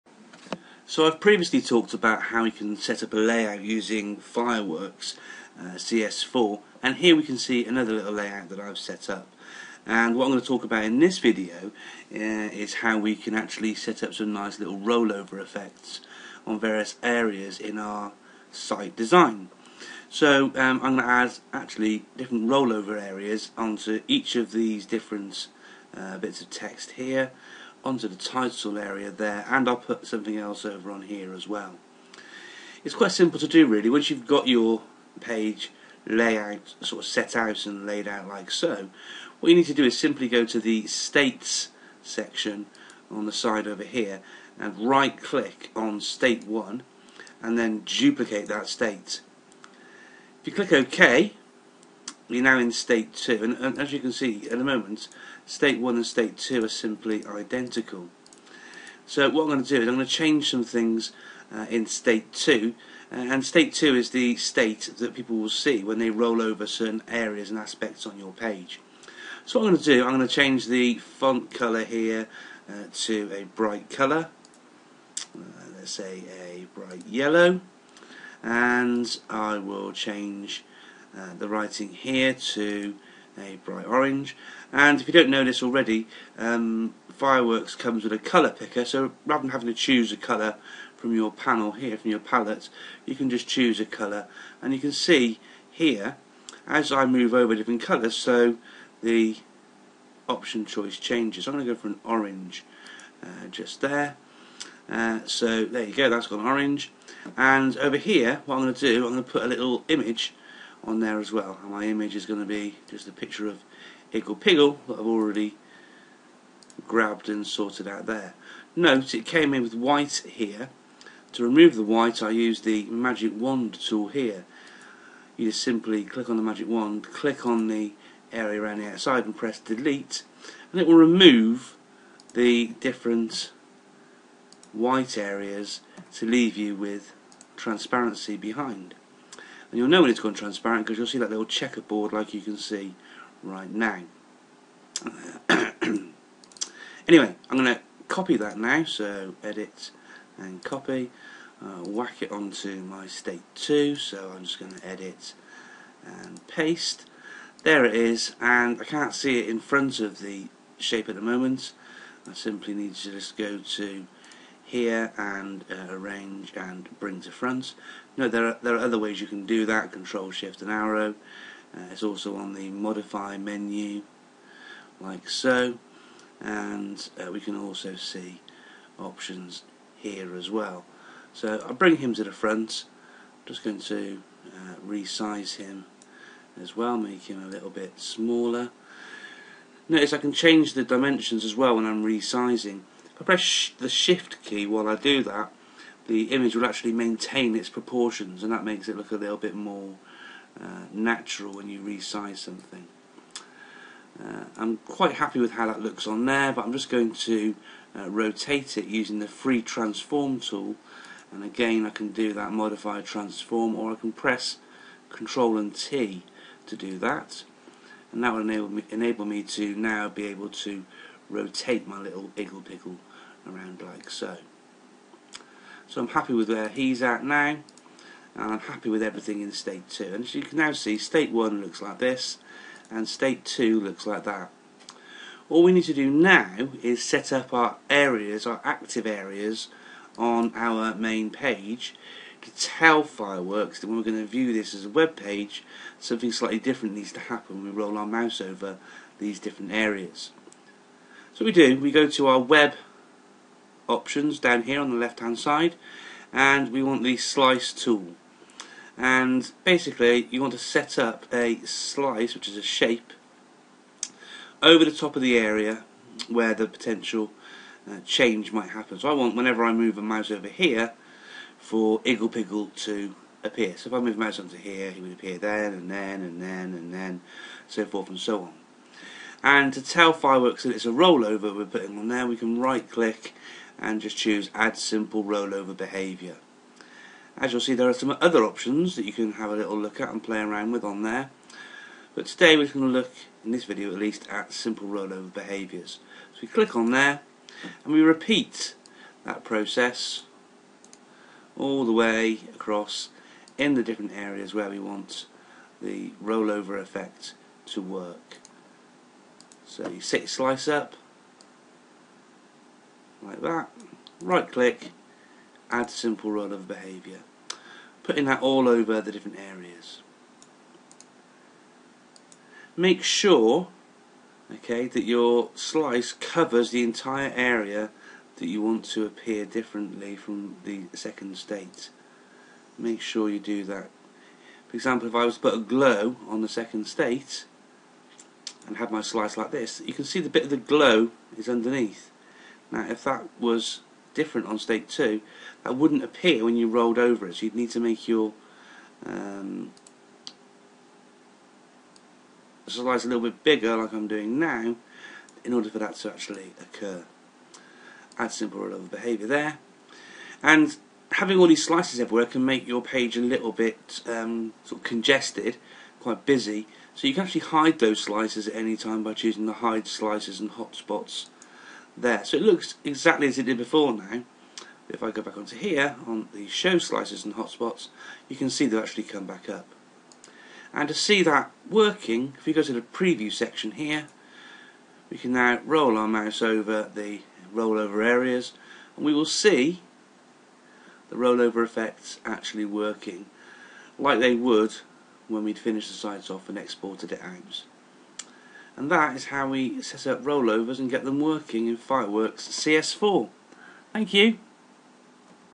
Making rollovers in Fireworks CS4
A short instructional video on how to make rollovers in Fireworks CS4